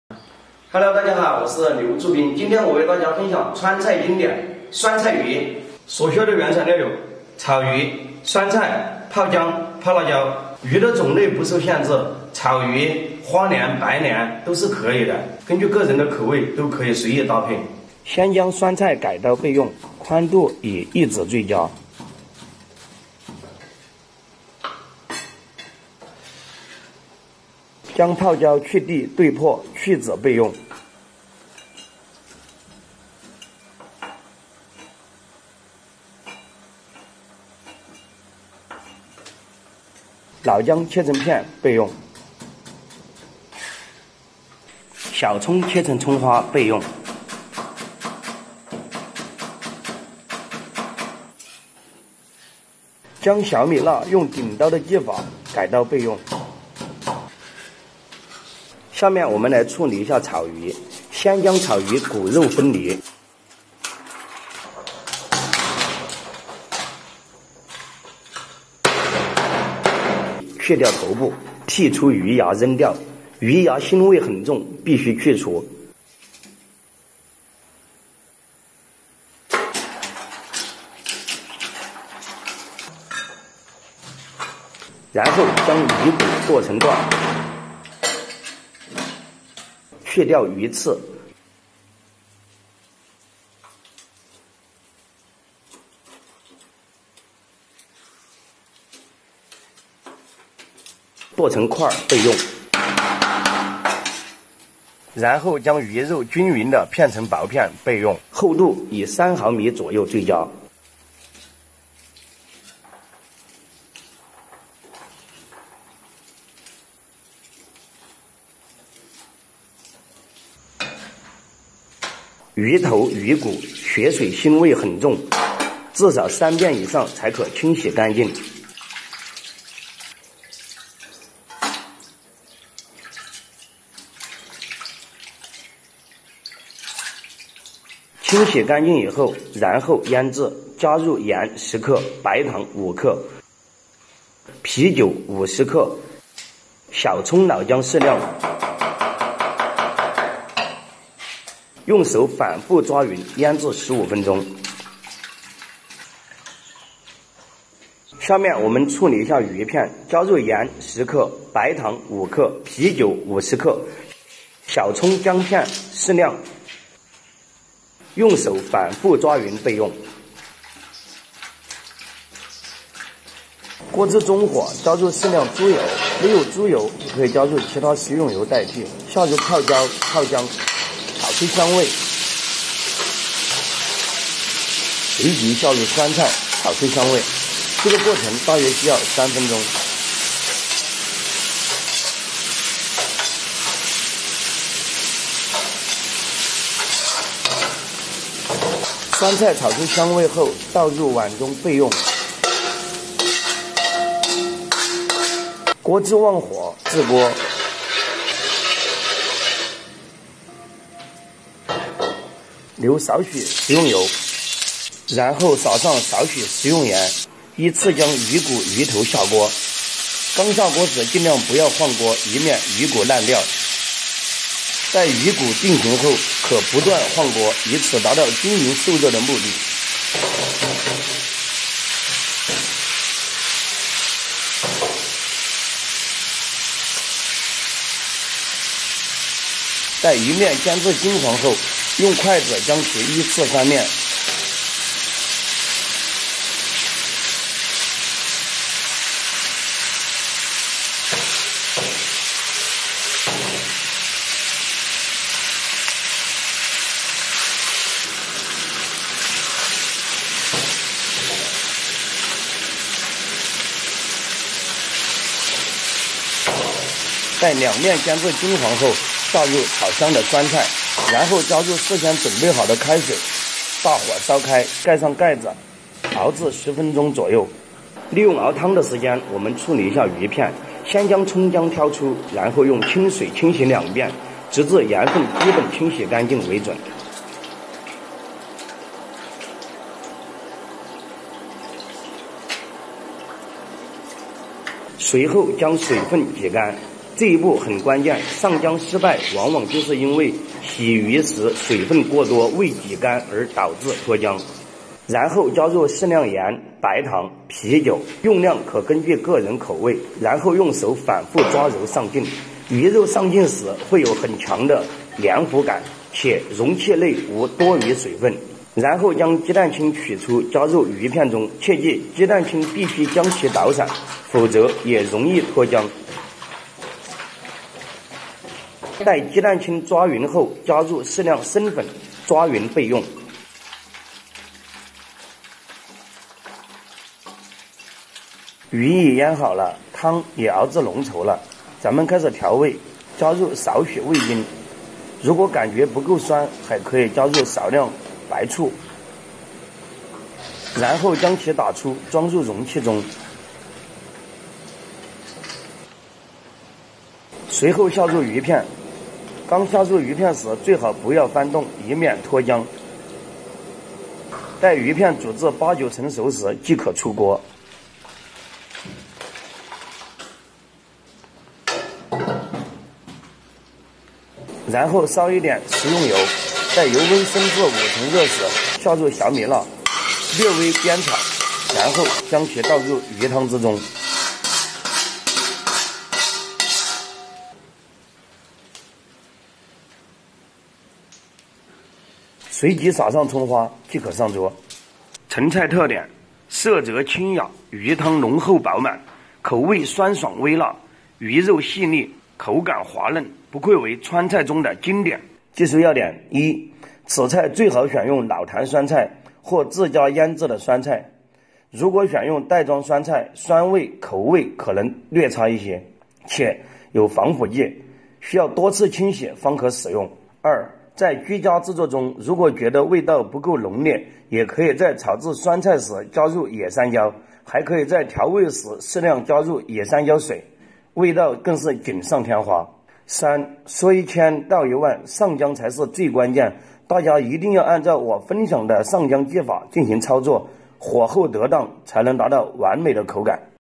国宴弟子分享“酸菜鱼的正宗做法”肉质滑嫩，口味酸爽，汤汁浓厚
在家也能轻松做酸菜鱼